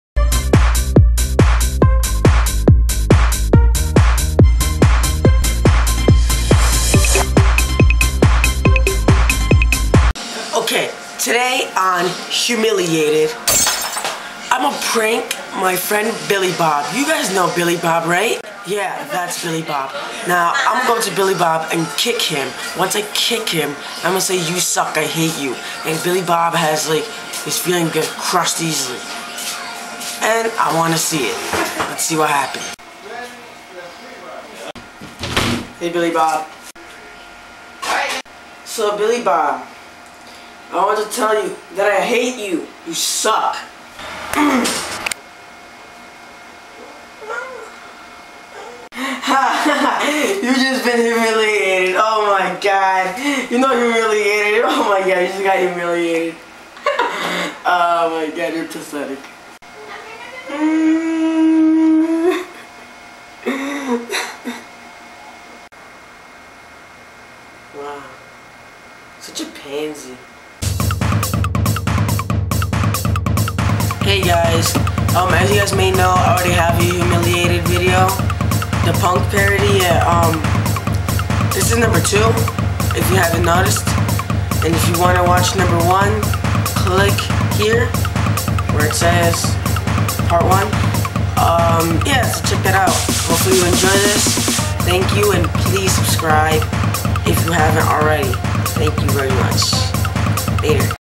HUMILIATE'D!PUNK'D Parody(2)
Here i make another video of HUMILIATE'D because i liked how the other one came out so i made another one..hopefully ima make a part 3...probally..lol...i prank my friend billy bob and i tell he sucks and i hate and he ends up crying

ALL SONGS IN THE VIDEO ARE MY SONGS I HAVE FULL COPYRIGHTS.! I MADE THE SONGS IN FL STUDIO...THE SOUND EFFECT IN THE VIDEO THAT SOUNDS LIKE A CRASHING NOISE S FROM FL STUDIO..